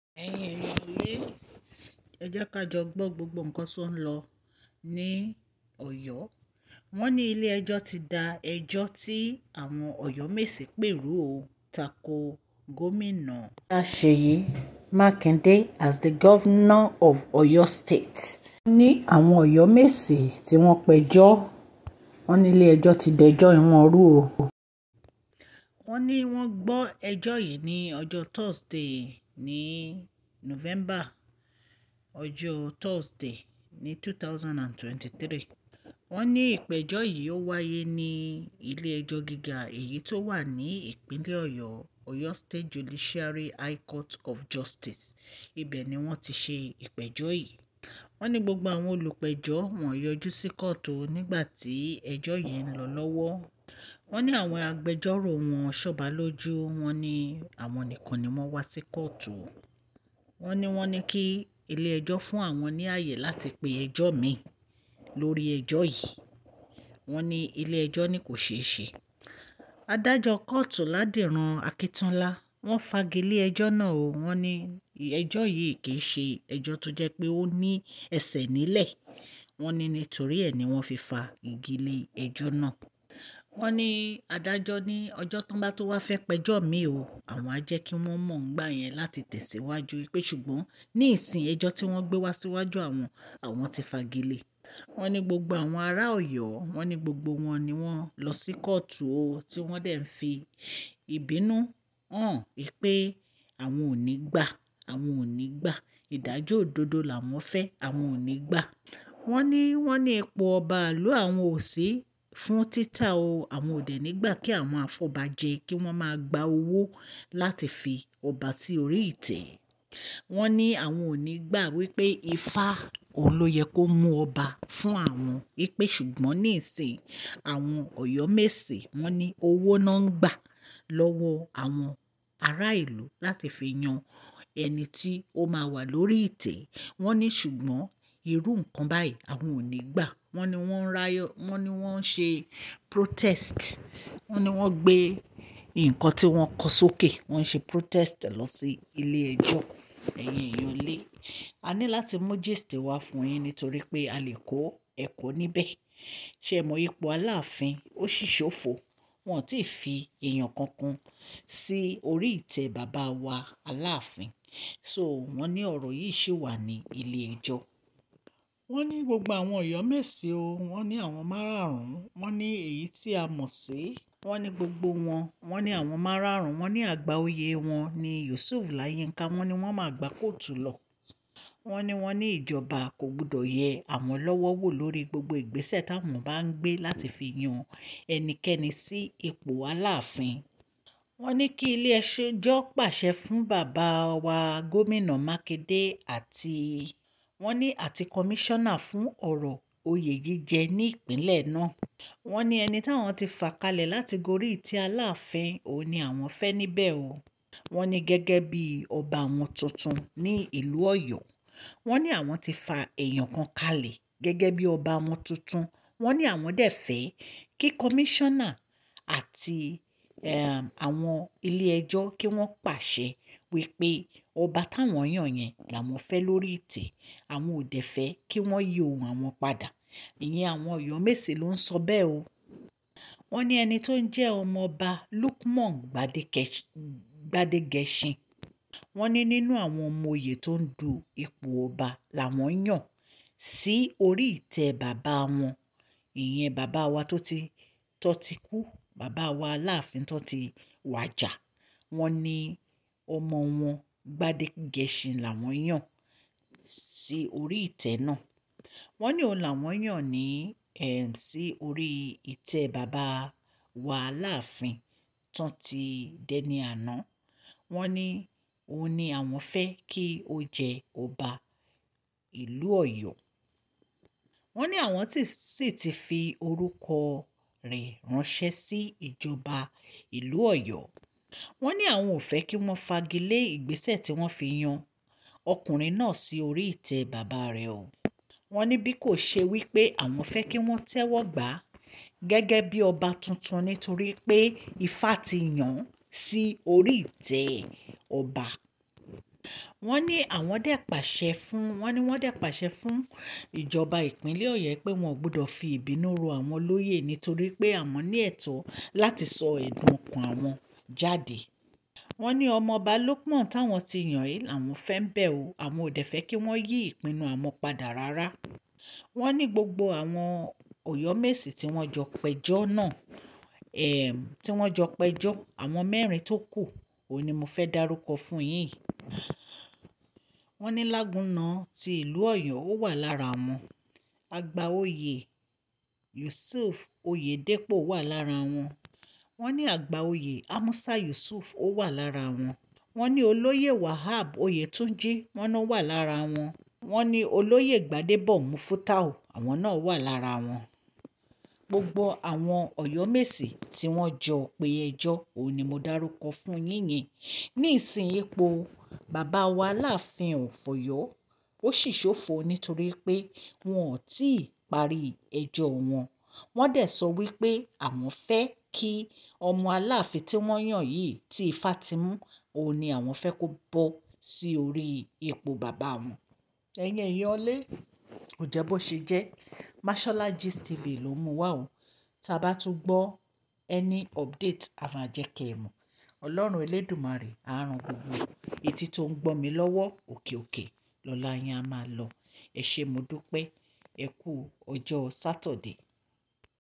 ILE EJO DA EJO TI AWON OYOMESI PE NU
The judiciary high court  dismiss the OYOMESI case because they didn't show up and they find no stagable case in it